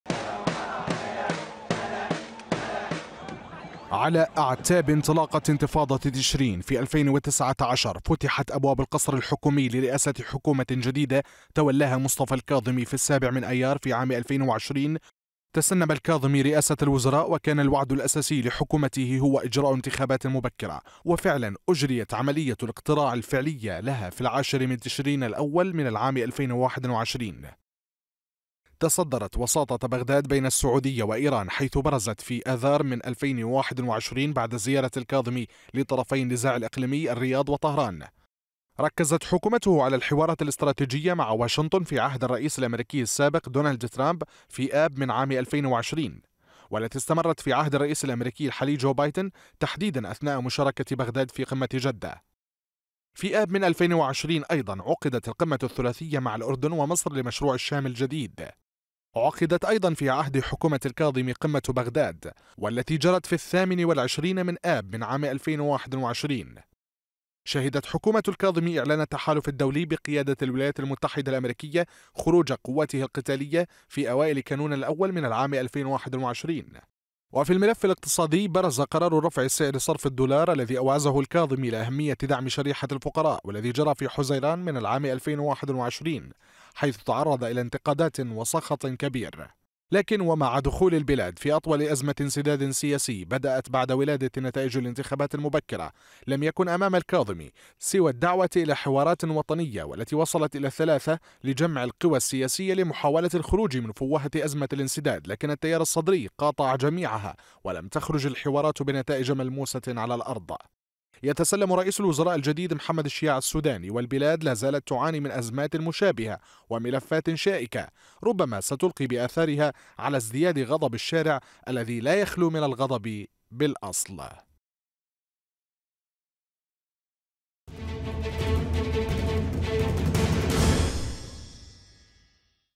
العراق في عهد حكومة الكاظمي.. هذا ما جرى ! | تقرير
على مدار قرابة عامين ونصف فترة ترؤس مصطفى الكاظمي مجلس الوزراء في الحكومة التي تشكلت عقب انطلاقة احتجاجات تشرين في 2019، برزت ملفات عدة واجهها الكاظمي وأنجز بعضا منها وشهدت حكومته قضايا مهمة فيها إلى أن وصلت البلاد إلى أزمة انسداد سياسي يعتبر الأطول في البلاد وكان الكاظمي قد دعا لحوارات وطنية لمحاولة إنهائه، إلى أن تم تكليف السوداني بتشكيل حكومة جديدة تواجه في مسيرتها ملفات سياسية واقتصادية صعبة.

موقع قناة التغيير الفضائية الالكتروني